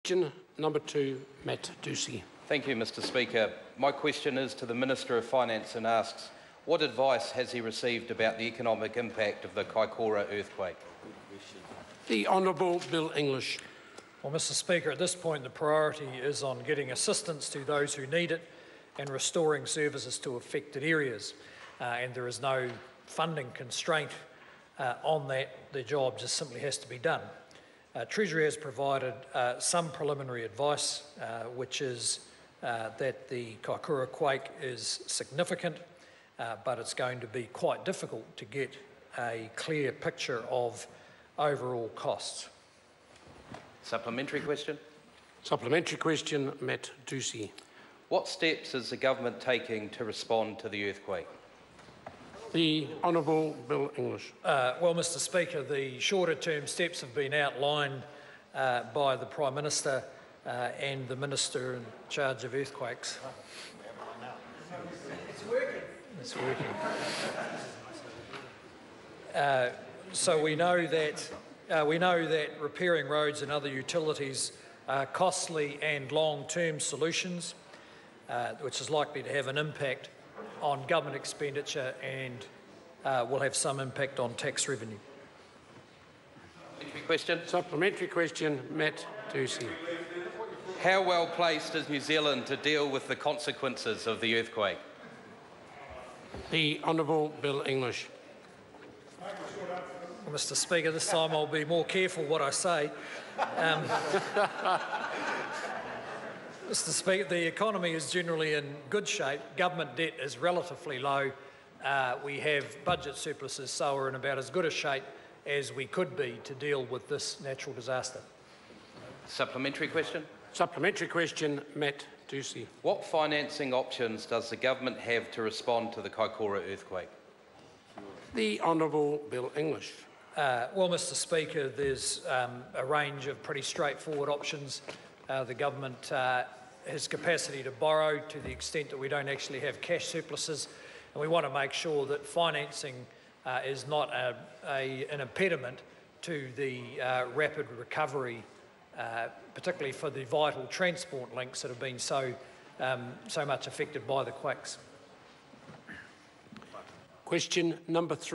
15.11.16 - Question 2 - Matt Doocey to the Minister of Finance
What advice has he received about the economic impact of the Kaikōura earthquake?